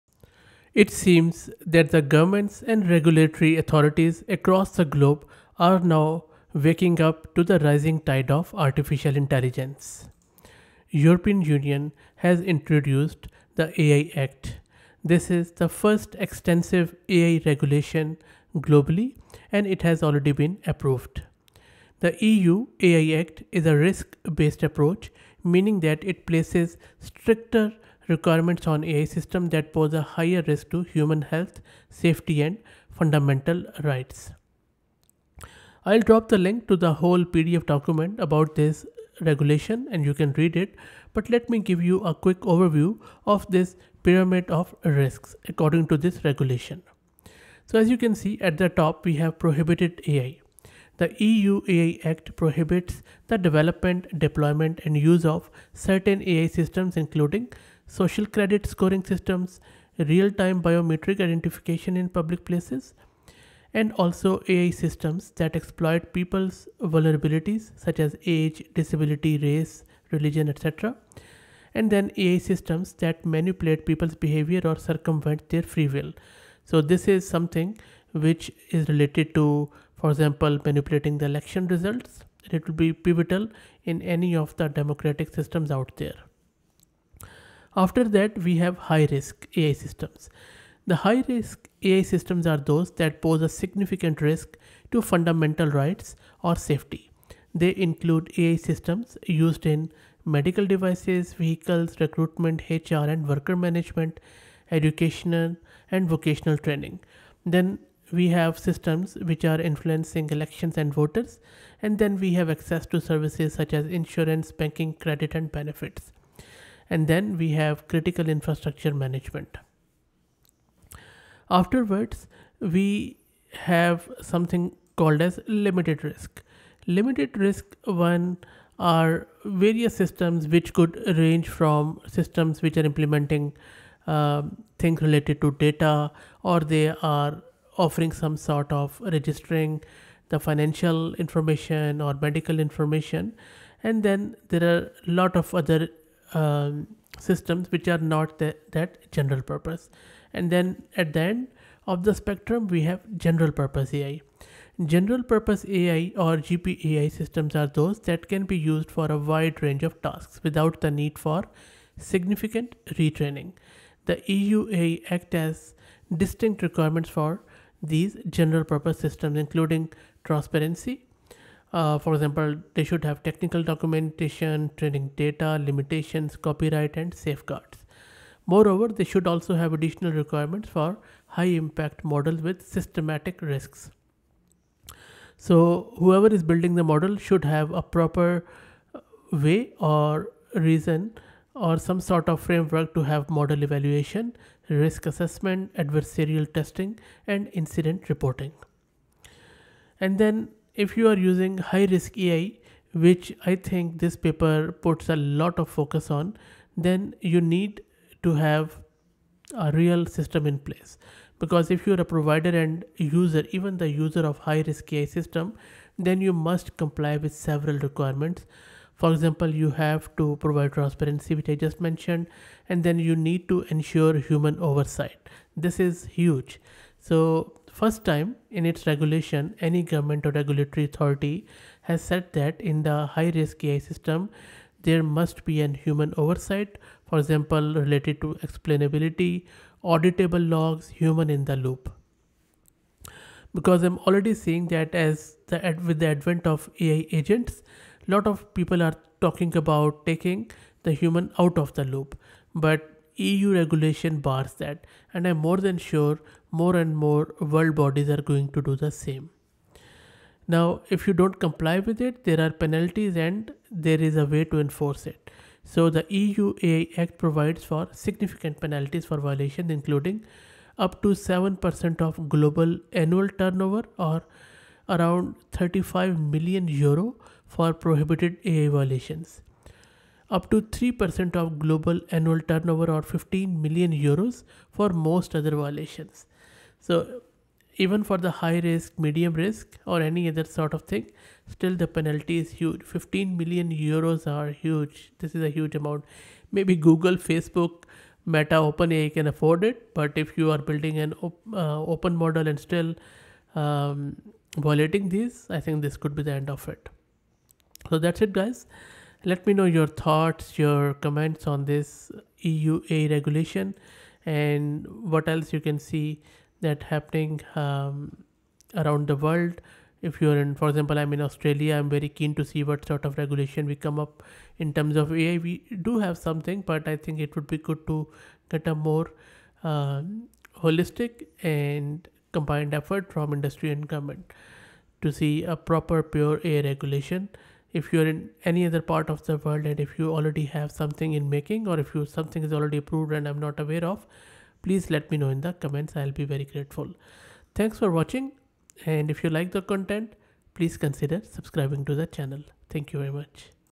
EU AI Act - AI Regulations Coming
This video reviews The EU AI Act which is a risk-based approach, meaning that it places stricter requirements on AI systems that pose a higher risk to human health, safety, and fundamental rights.